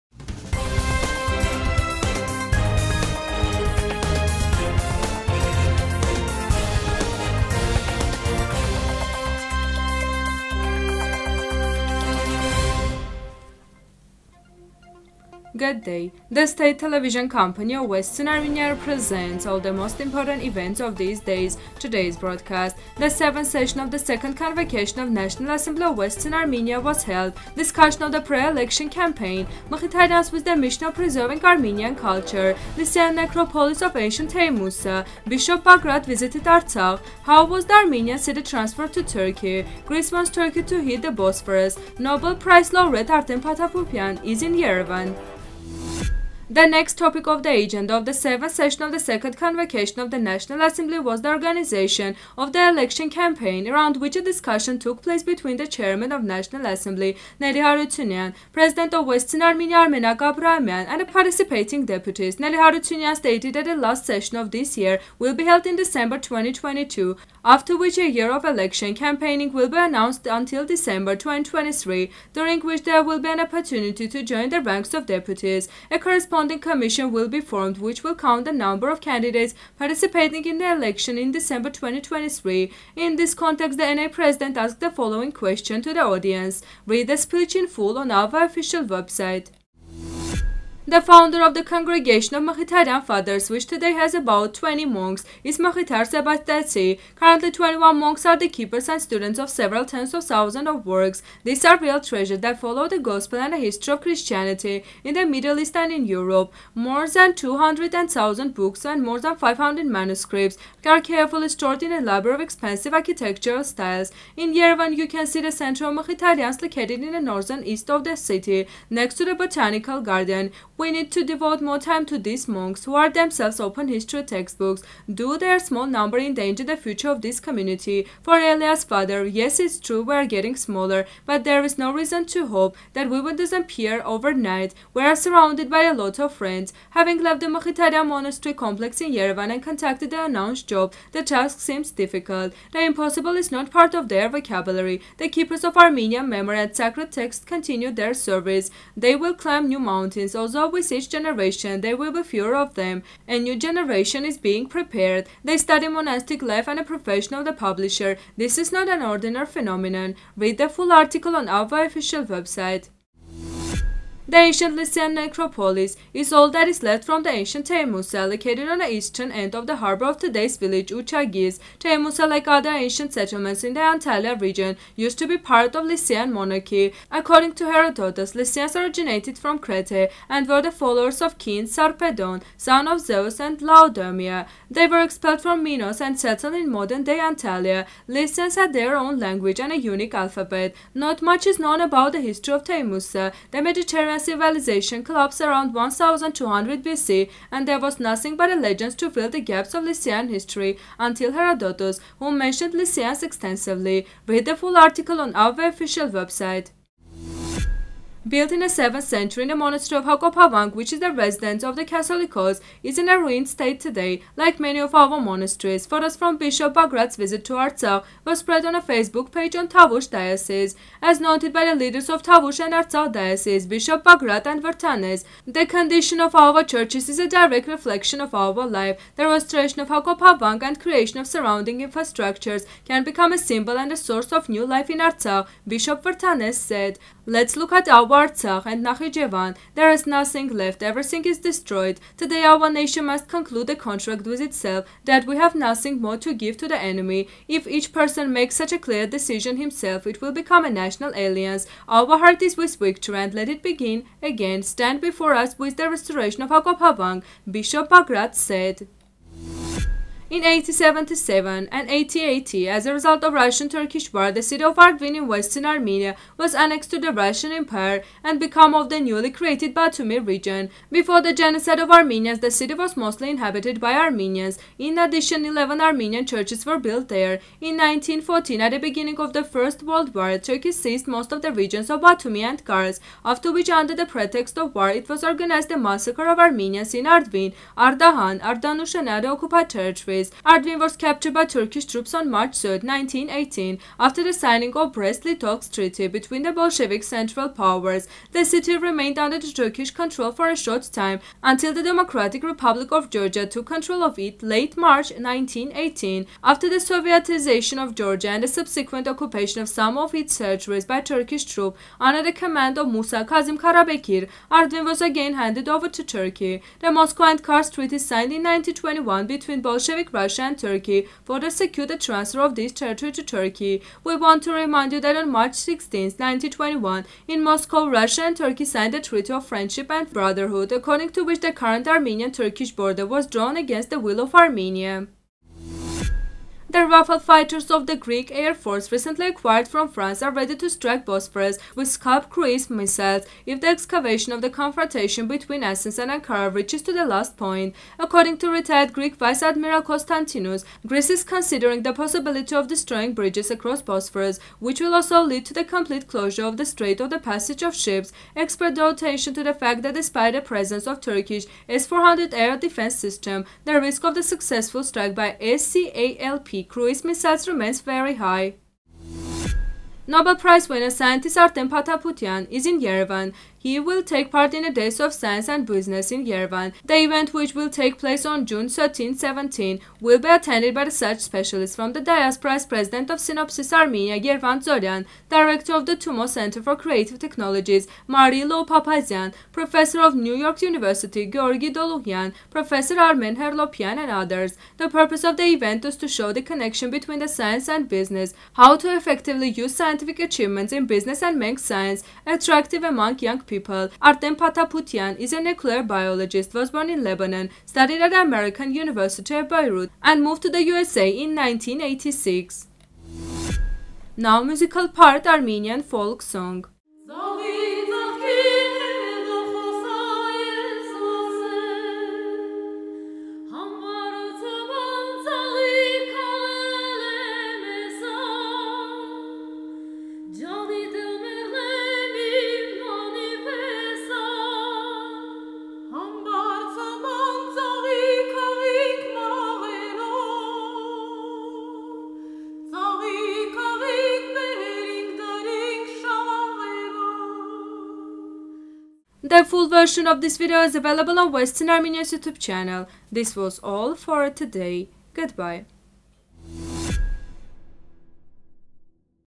The Mkhitaryans with the mission of preserving Armenian culture
1. The 7-th session of the 2nd convocation of the National Assembly of Western Armenia. discussion of the pre-election campaign
2. The Mkhitaryans with the mission of preserving Armenian culture
3. The Lycian necropolis of ancient Teimiussa
4. Bishop Bagrat visited Artsakh
5. How was the Armenian city transferred to Turkey?
6. Greece warns Turkey to  hit the Bosphorus
7. Nobel Prize laureate Artem Pataputyan is in Yerevan